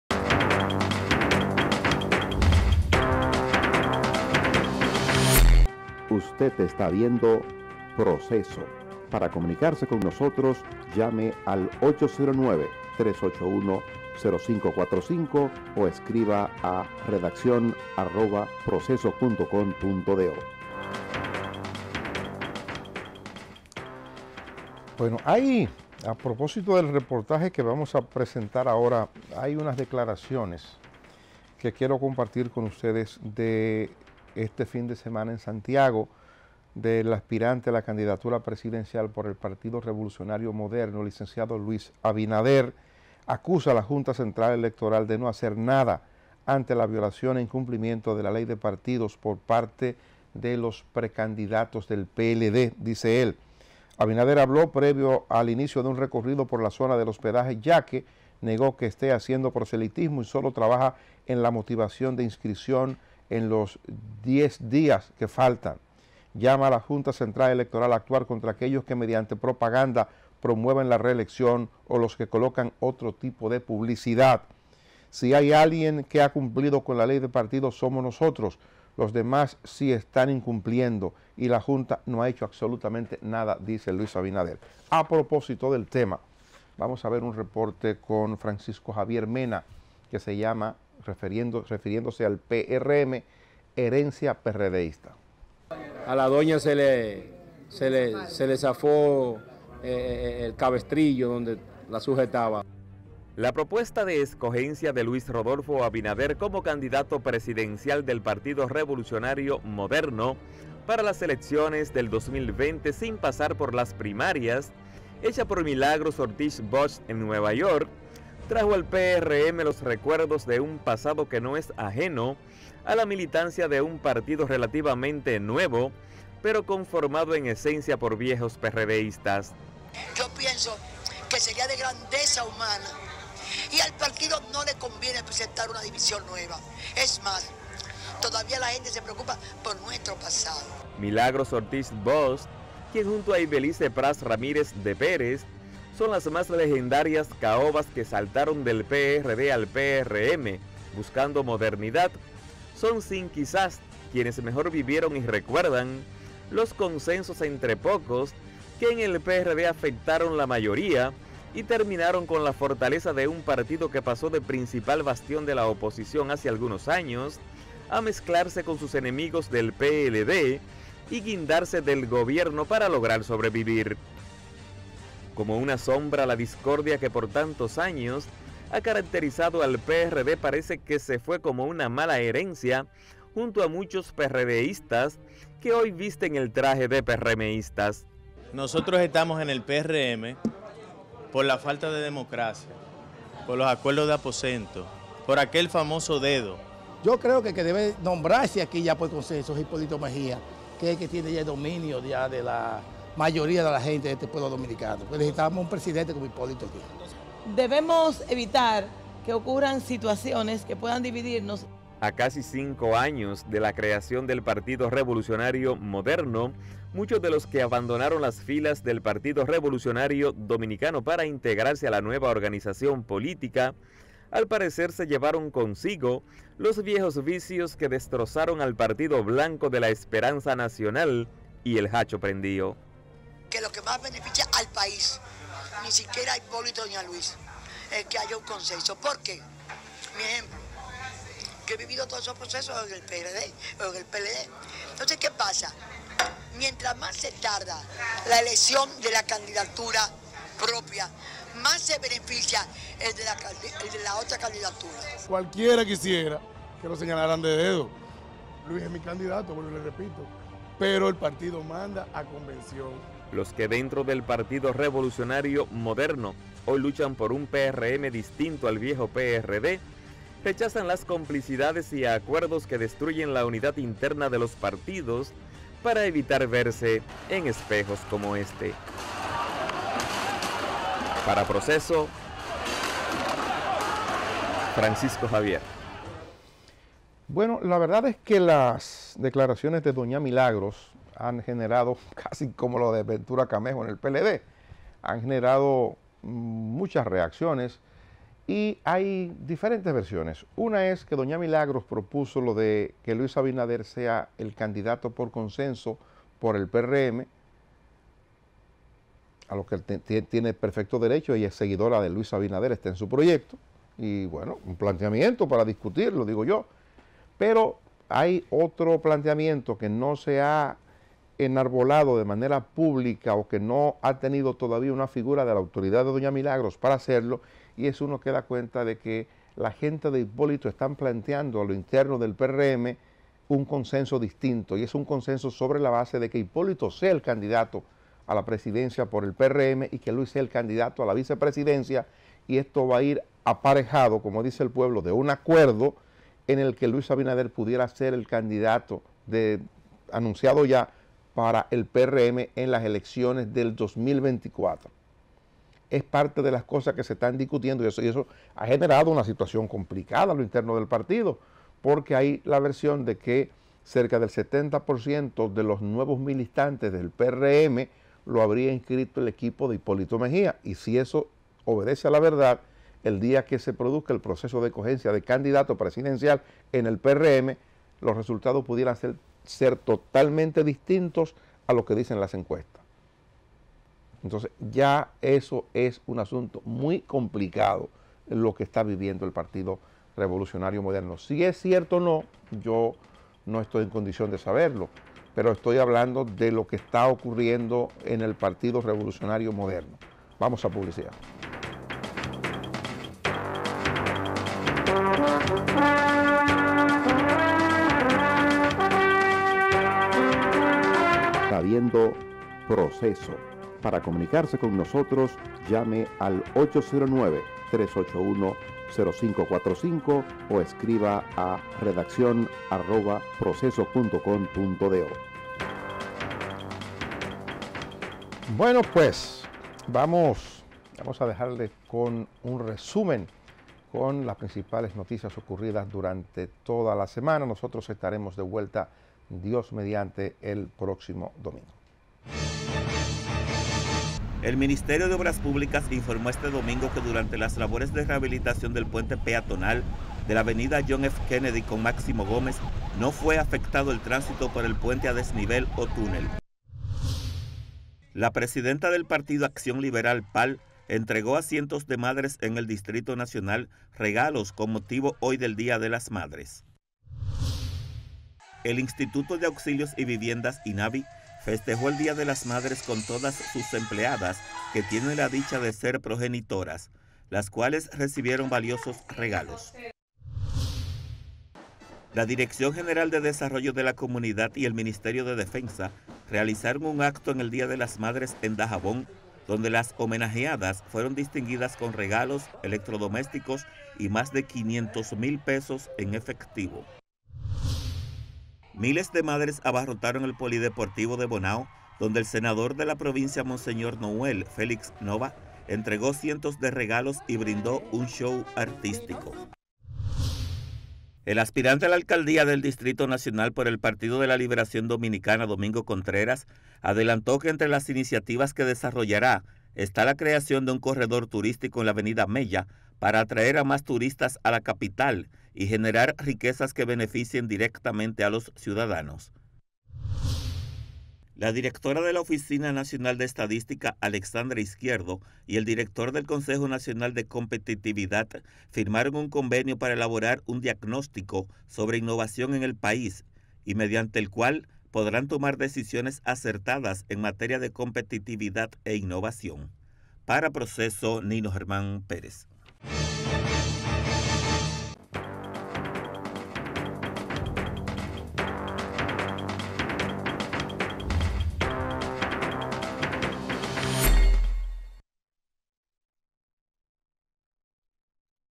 EN EL PRM TAMBIÉN SE CUECEN HABAS | PROCESO III
Doña Milagros propuso que por consenso se escoja a Luis Abinader como candidato a la presidencia y de inmediato surgieron reacciones, inclusive de irrespeto hacia la exvicepresidenta de la República.